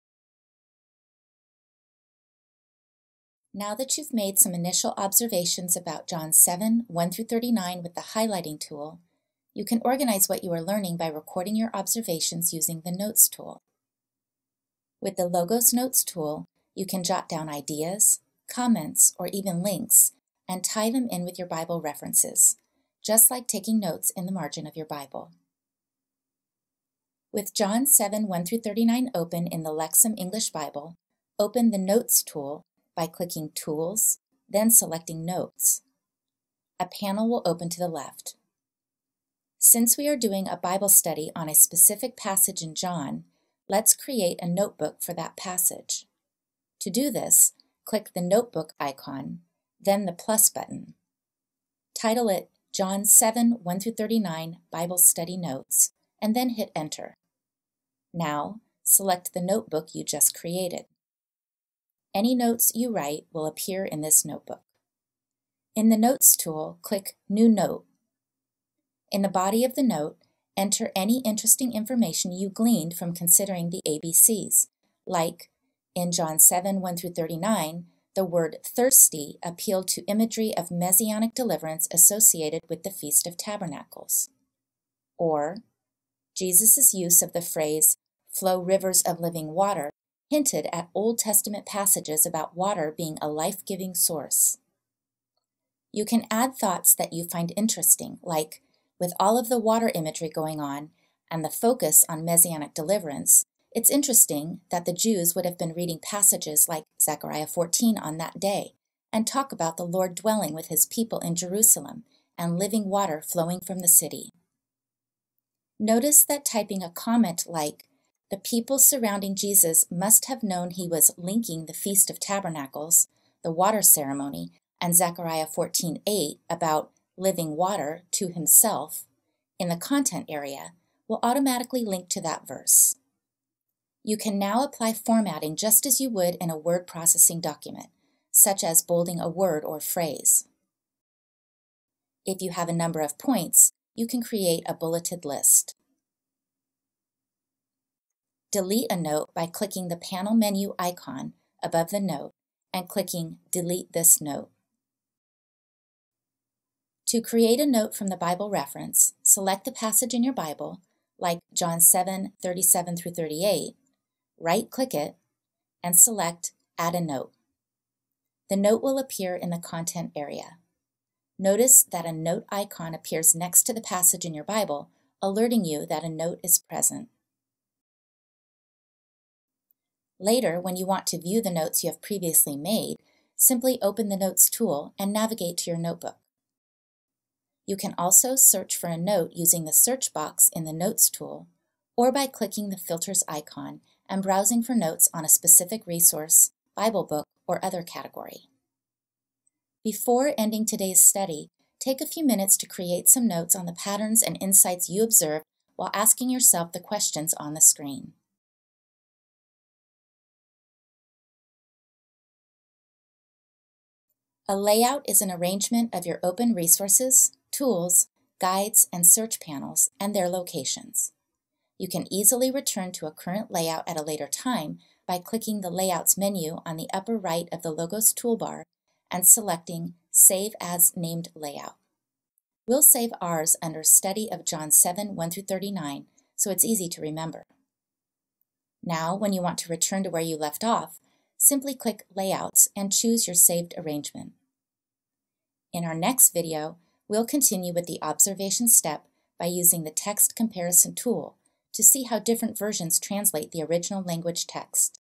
Observation: Notes Tool | Logos Bible Software
Learn how to record your study observations using the Notes tool in Logos.